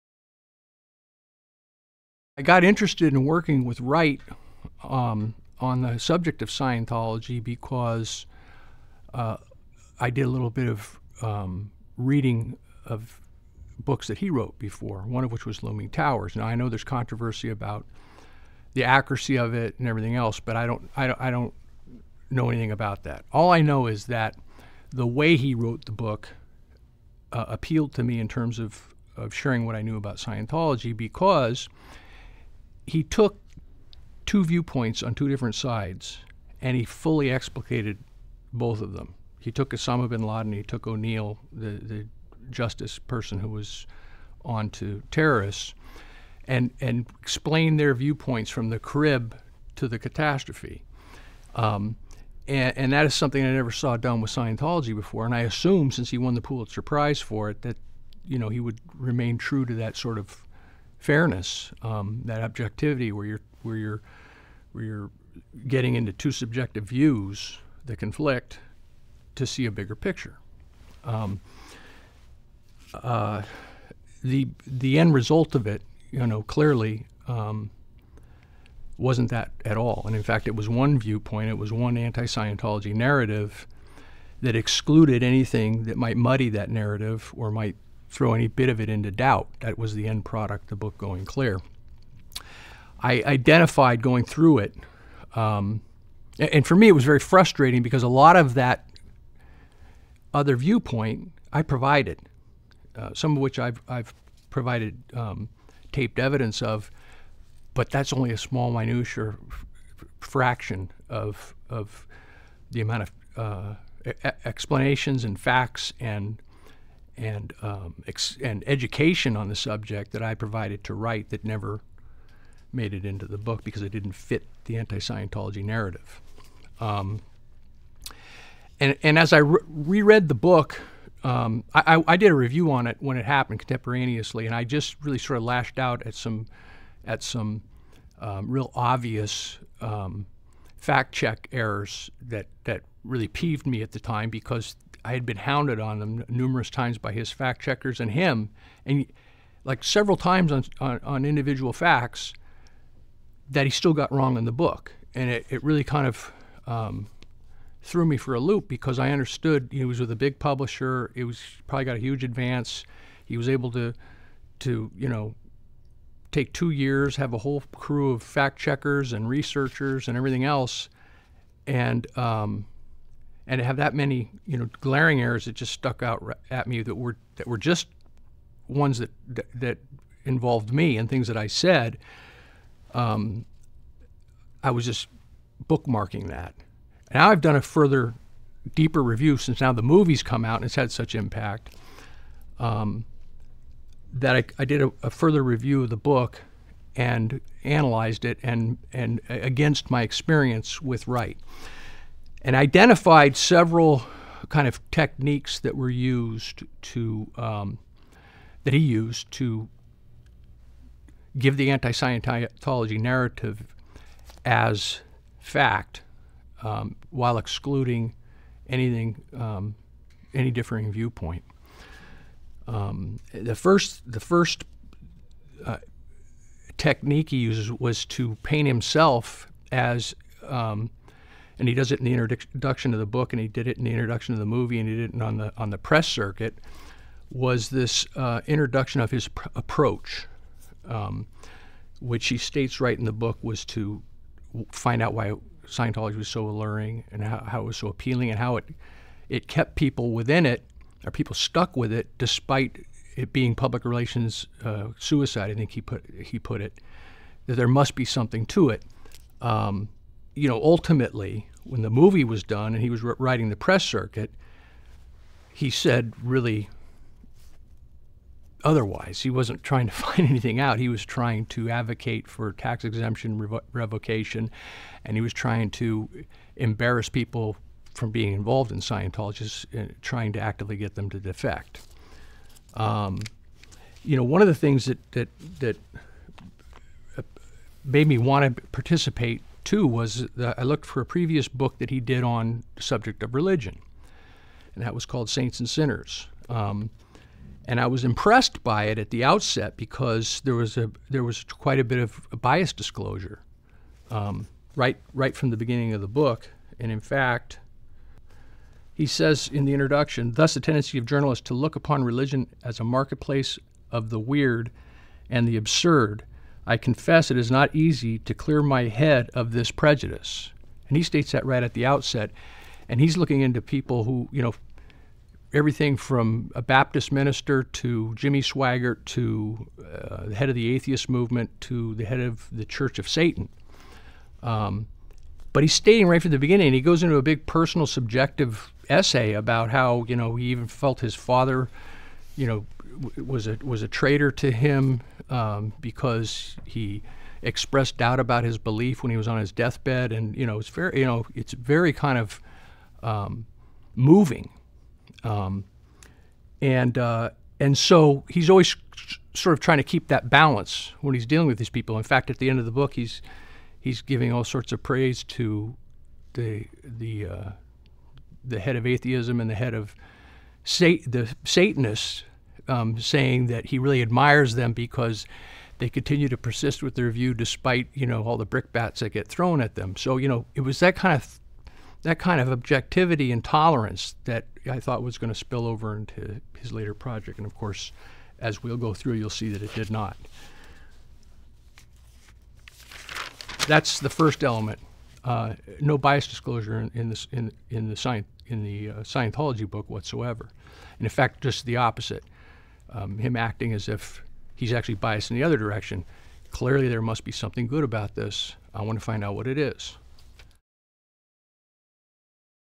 Going Clear, Part 1 Lawrence Wright, False Representations about himself and motives
Analysis of the book Going Clear by Lawrence Wright, Part 1.  No Bias Disclosure.